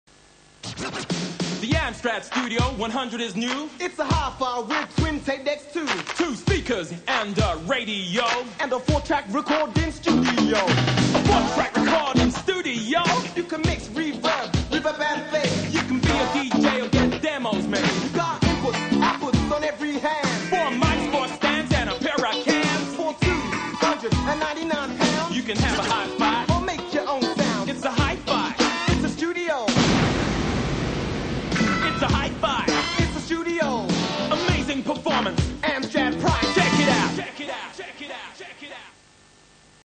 Amstrad Advert c. 1985
Amstrad home studio/hi-fi. An awesome rap on this boasting its mixing credentials. The only part I remember was the exploding hi-fi but in reverse! How did they do that?